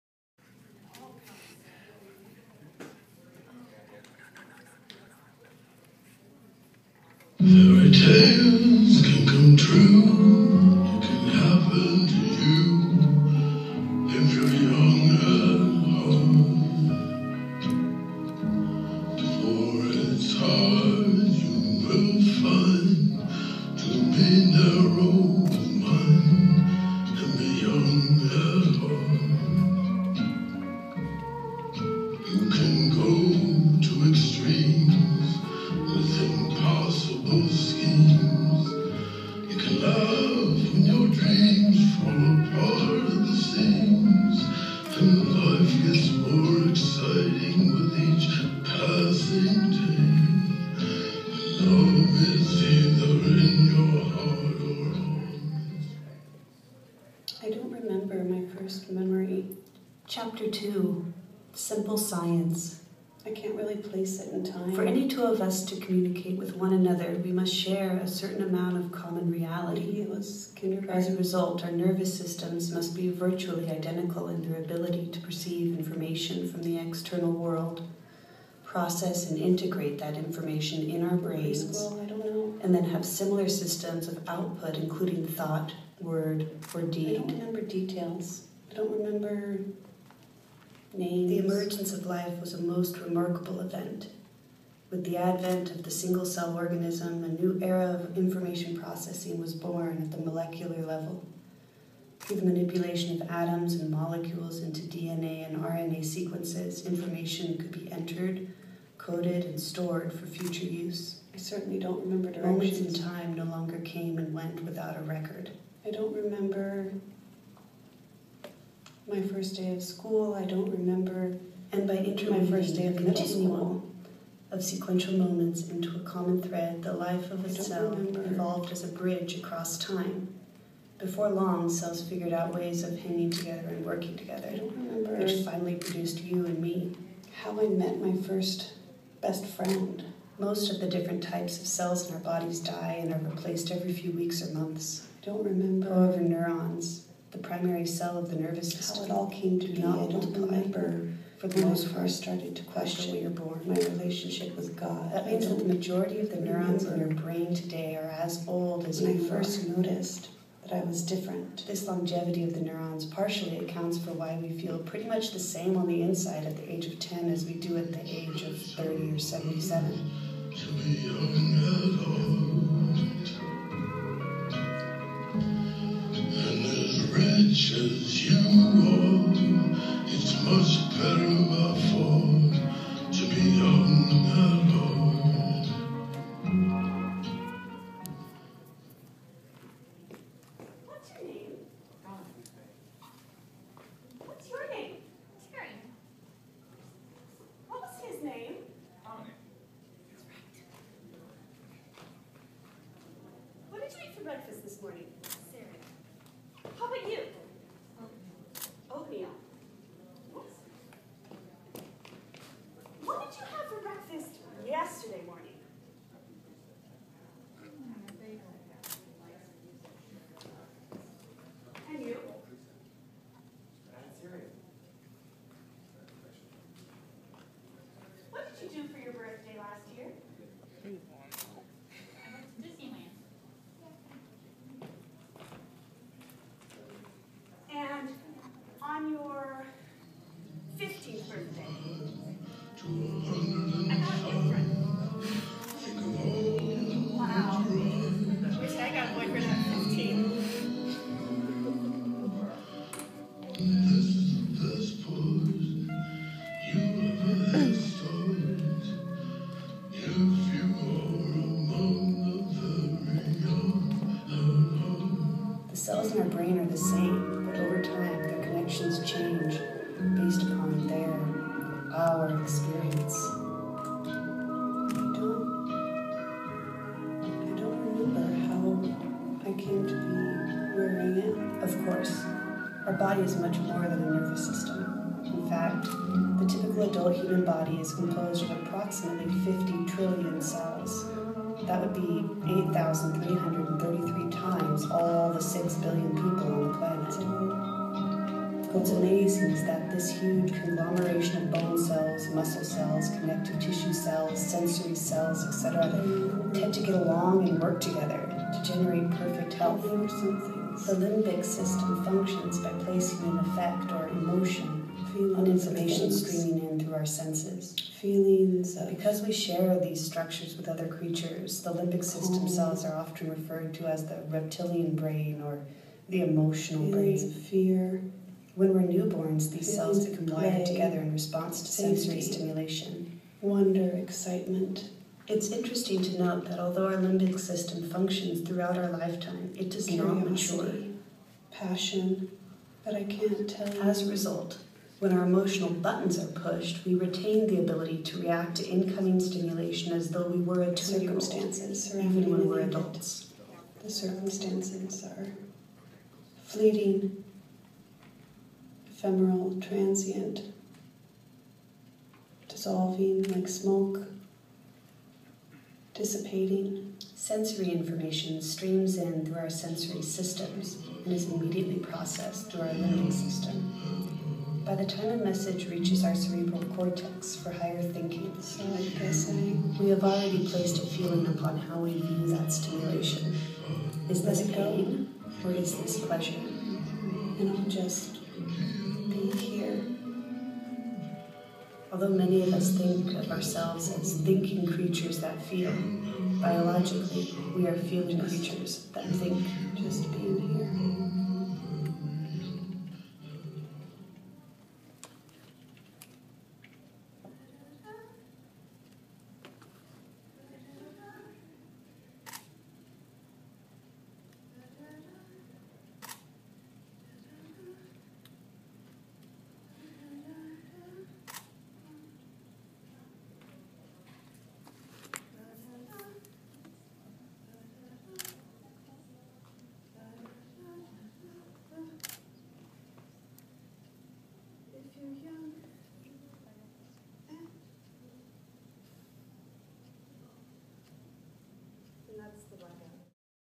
Inside the Vault: Megill & Company (First Dress 8-13)
Beth Megill of Megill & Company, performs her newly conceived structured improvisation, Inside the Vault, based on growing up, growing old, making memories and forgetting them.